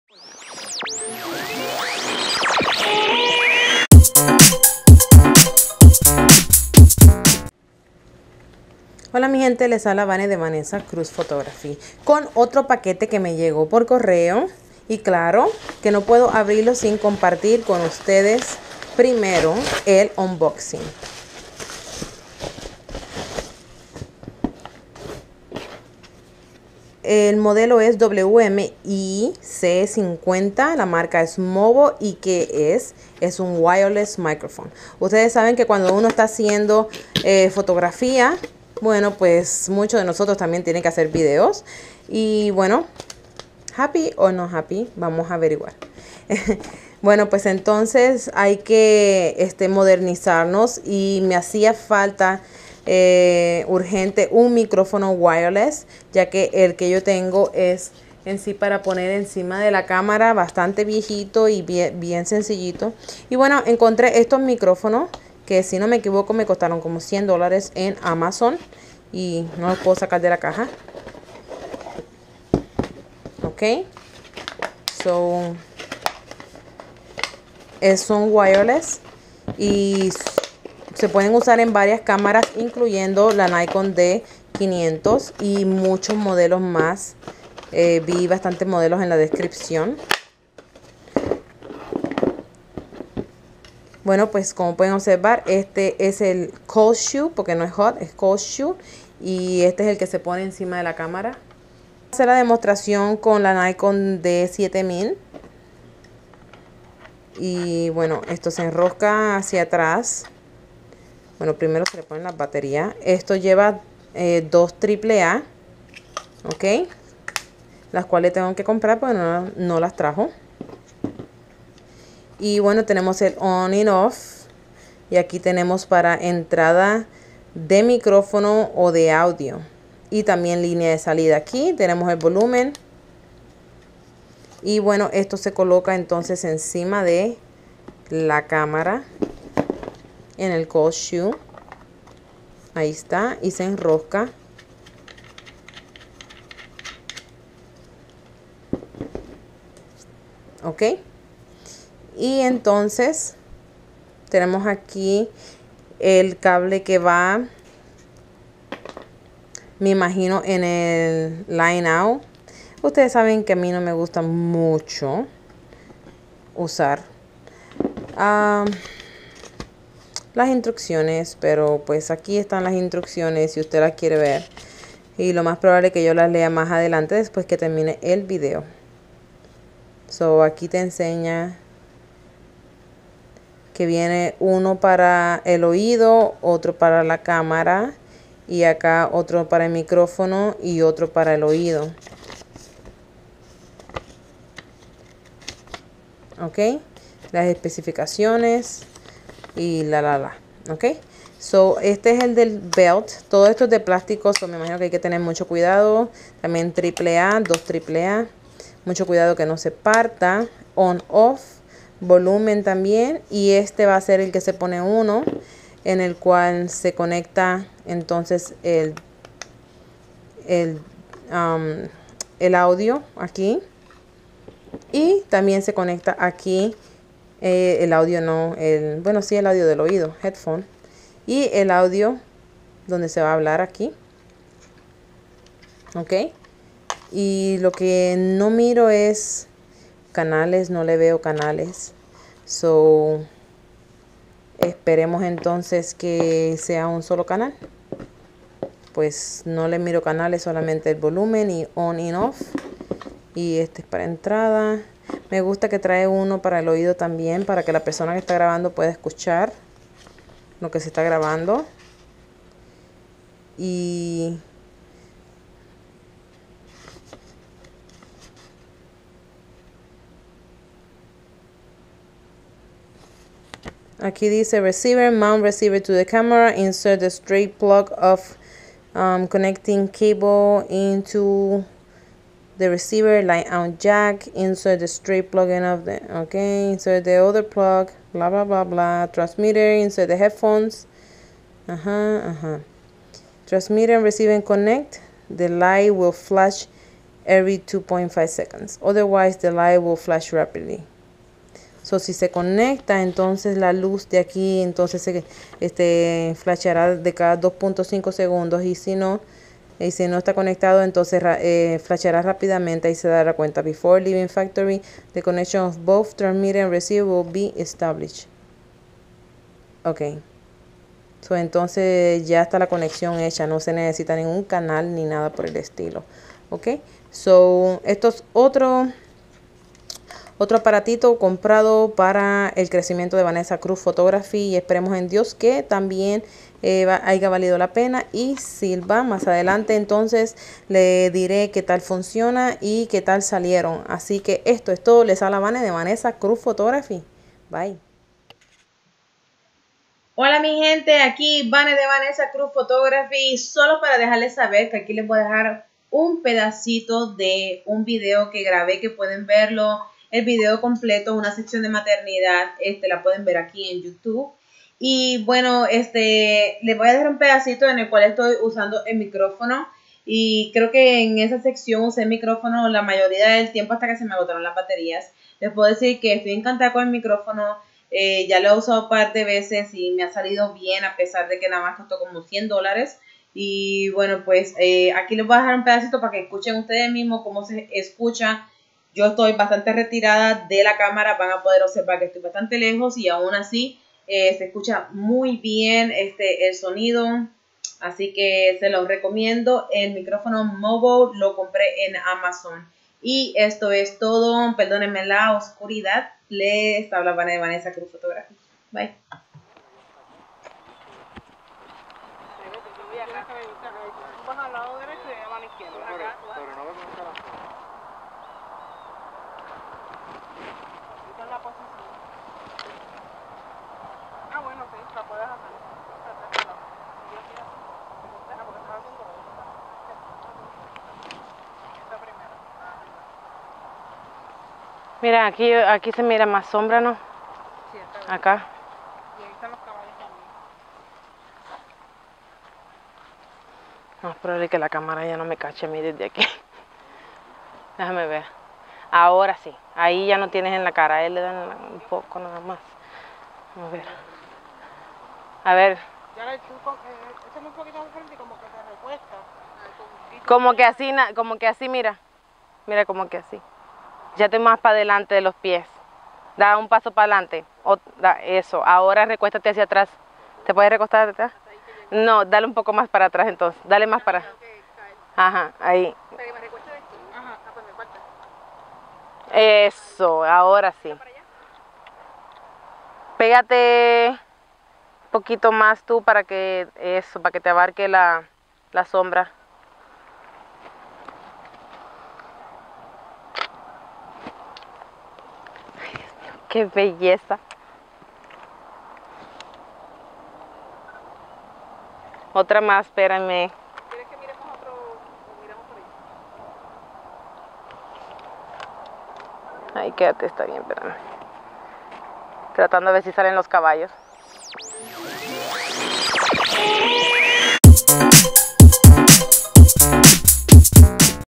Unboxing El Microfono Inalambrico Movo En Espanol Por Vanessa Cruz Photography
Aqui les dejo in videito unboxing in Nuevo microfono inalambrico que compre para mis grabaciones. Les digo que me lo lleve para Michoacán Mexico y Tambien en una Sesion de Maternidad y ahi les dejo in pedacito para qeu vean que tal se oye!

Ya saben que les agradesco su subcripcion a mi canal y
in Like a mi Paginita en Facebook